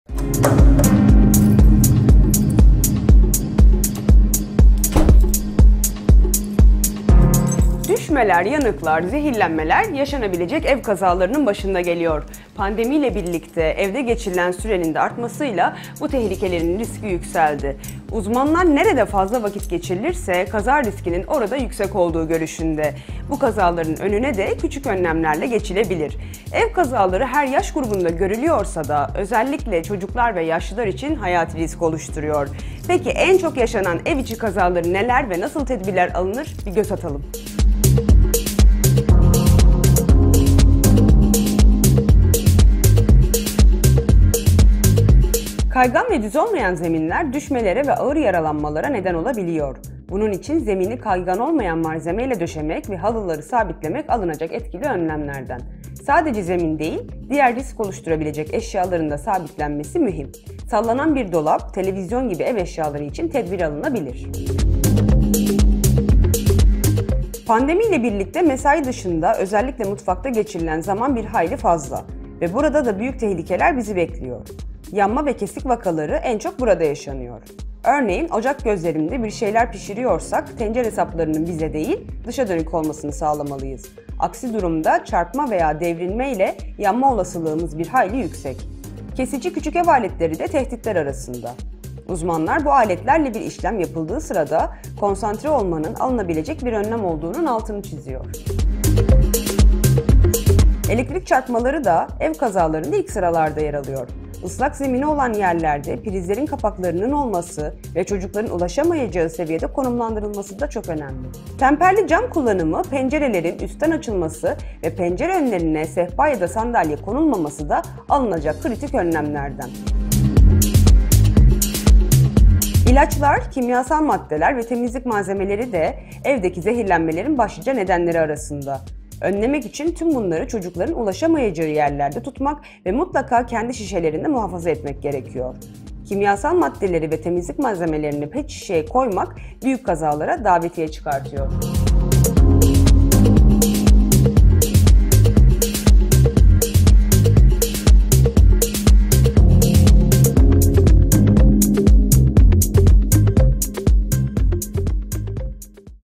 Tehlikeli ev kazalarının önüne nasıl geçeriz?
Düşmeler, yanıklar, zehirlenmeler evde yaşanabilecek kazaların başında geliyor. Pandemide evde geçirilen sürenin artmasıyla da söz konusu tehlikelerin riski yükseldi. Oysaki bu kazaları basit tedbirlerle önlemek mümkün.